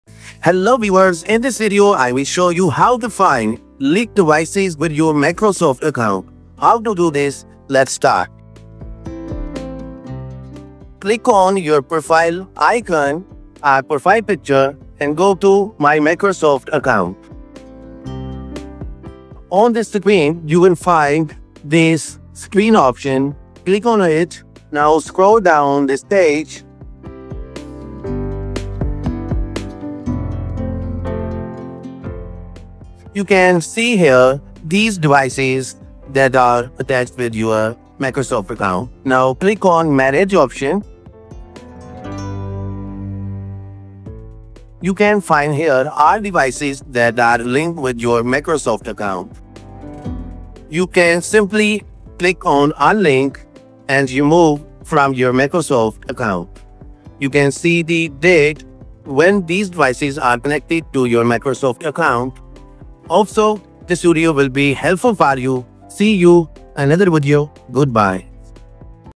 🔍 How to Find Linked Devices with Your Microsoft Account | 2025 Guide
Want to check which devices are linked to your Microsoft account? In this video, I’ll show you how to easily view and manage all connected devices, including Windows PCs, Xbox consoles, and mobile devices.

✨ Steps to Find Linked Devices:
    Go to Microsoft Account Page
        Visit account.microsoft.com.
    Sign In
        Use your Microsoft account credentials to log in.
    Access Devices Section
        Click on the ‘Devices’ tab from the top menu.
    View Linked Devices
        A list of all your registered devices will appear.
    Manage or Remove Devices
        Click on each device to view details, manage, or unlink it if necessary.

🔧 Why Check Linked Devices?

    Security – Remove old or unauthorized devices.
    Account Management – Track how many devices use your account.
    Subscription Control – Monitor Microsoft 365 or software activations.

    How to Remove Devices from Microsoft Account
    How to Reset Microsoft Account Password
    Fix Device Not Syncing with Microsoft Account

💬 Found devices you didn’t recognize? Let me know how this guide helped!

Check our Products and support us